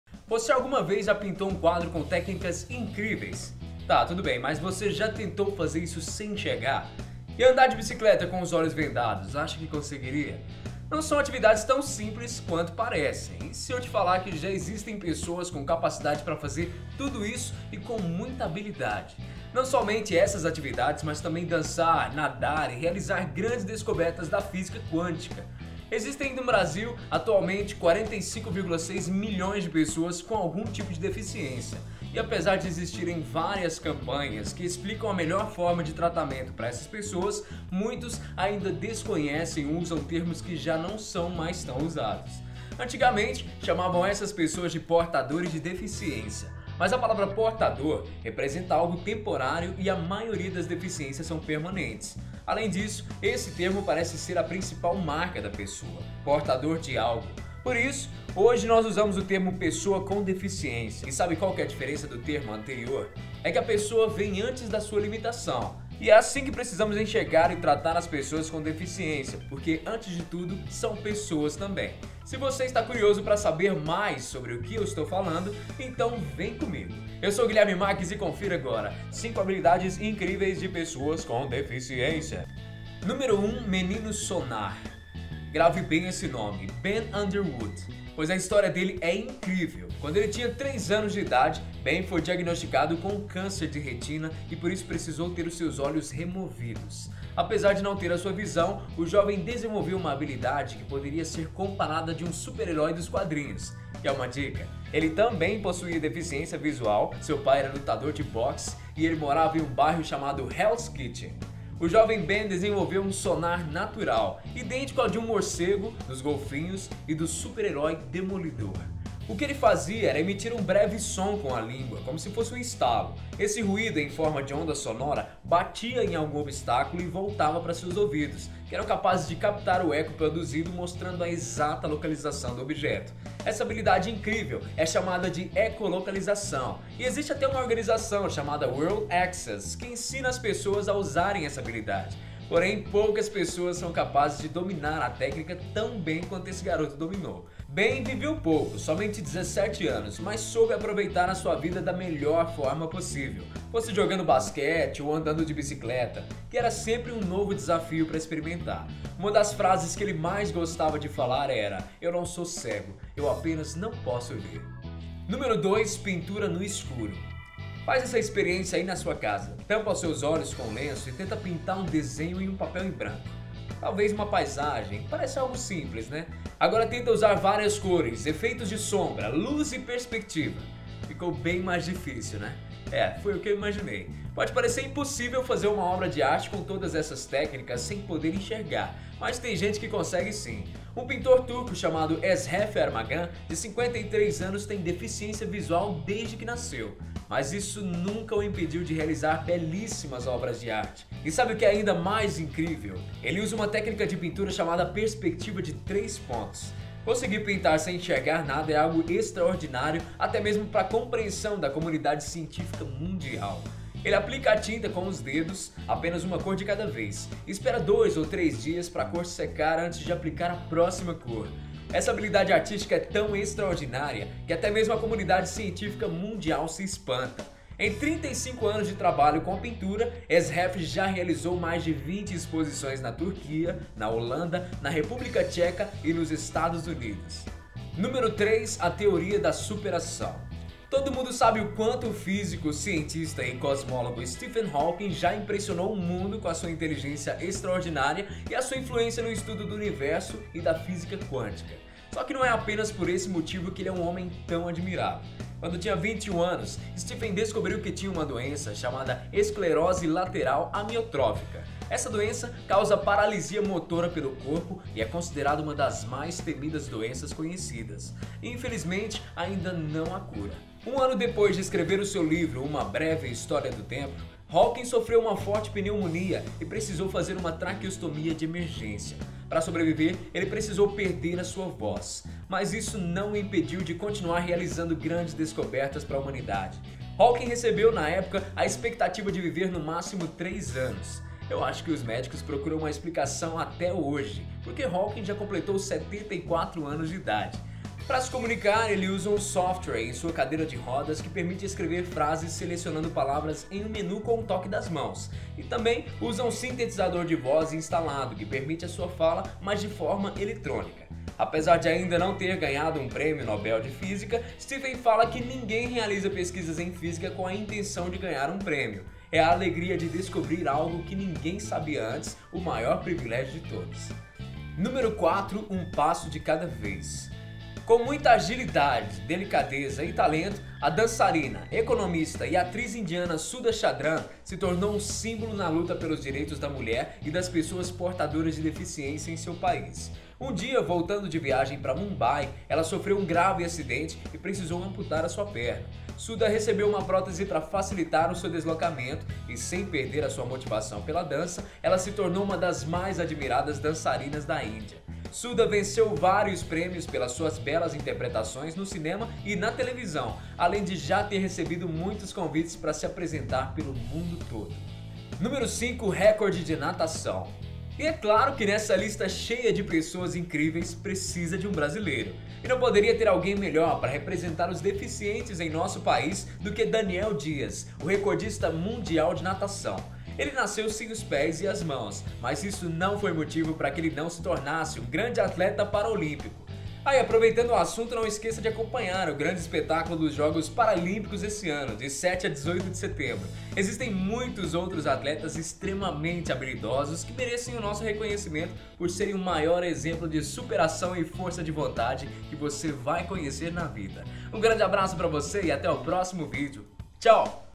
5 Habilidades Incríveis de Pessoas com Deficiência
Você alguma vez já pintou um quadro com técnicas incríveis? Tá, mas você tentou fazer isso sem enxergar? E andar de bicicleta com os olhos vendados, acha que conseguiria? Não são atividades tão simples quanto parecem.
Existem no Brasil, atualmente, 45,6 milhões de pessoas com algum tipo de deficiência. E apesar de existirem várias campanhas que explicam a melhor forma de tratamento para essas pessoas, muitos ainda desconhecem ou usam termos que já não são mais tão usados.
Antigamente, chamavam essas pessoas de portadores de deficiência. Mas a palavra “portador” representa algo temporário e a maioria das deficiências são permanentes. Além disso, esse termo parece ser a principal marca da pessoa. Portador de algo.
Por isso, hoje nós usamos o termo pessoa com deficiência. E sabe qual é a diferença do termo anterior? É que a pessoa vem antes da sua limitação. E é assim que precisamos enxergar e tratar as pessoas com deficiência, porque, antes de tudo, são pessoas.